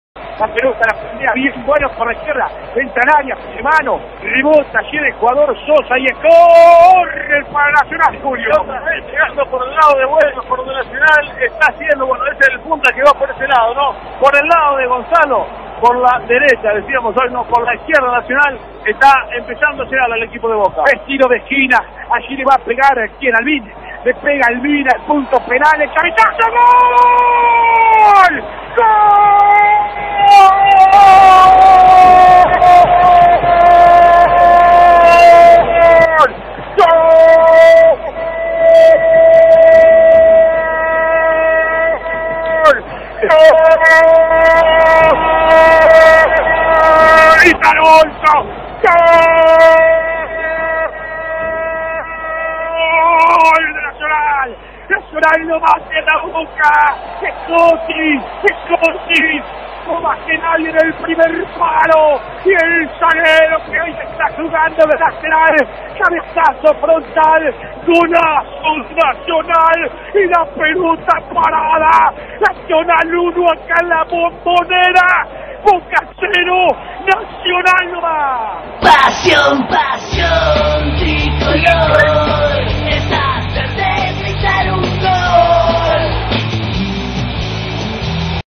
Boca 0 Nacional 1 (Audio Pasion Tricolor Radio) Copa Libertadores 2013 (7/3/2013)
El gol del partido con relato de Javier Moreira Picolor Radio Am Pasion Tricolor Am 1010